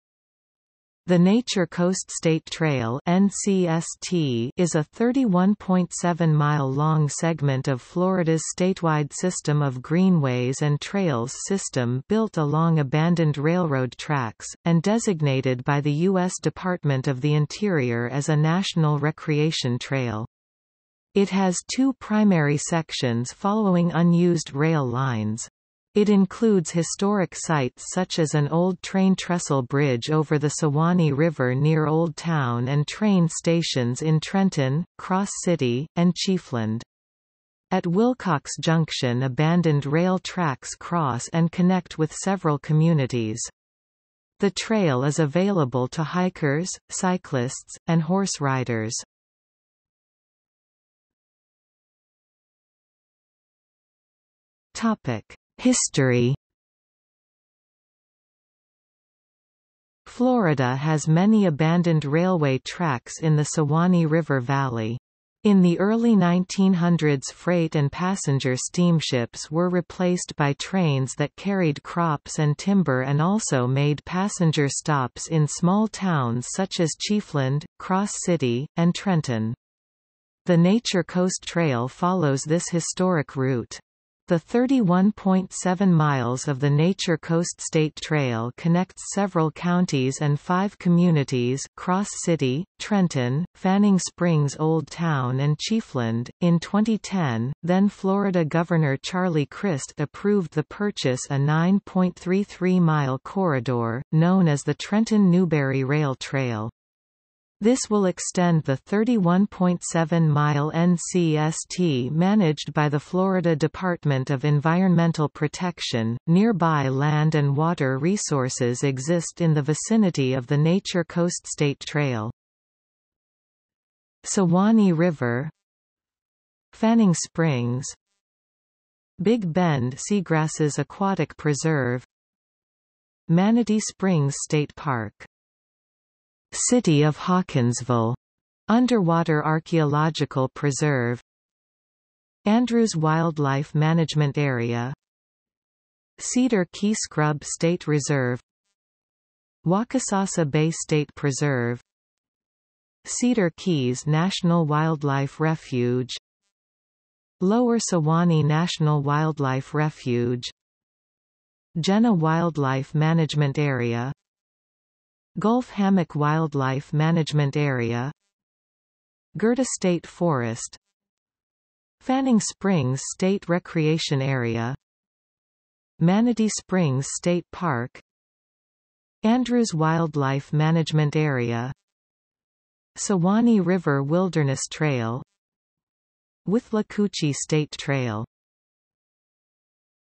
Nature Coast State Trail | Wikipedia audio article
00:00:51 History

- Socrates

SUMMARY
The Nature Coast State Trail (NCST) is a 31.7-mile long segment of Florida’s Statewide System of Greenways and Trails System built along abandoned railroad tracks, and designated by the U.S. Department of the Interior as a National Recreation Trail.  It has two primary sections following unused rail lines. It includes historic sites such as an old train trestle bridge over the Suwannee River near Old Town and train stations in Trenton, Cross City, and Chiefland.  At Wilcox Junction abandoned rail tracks cross and  connect with several communities. The trail is available to hikers, cyclists, and horse riders.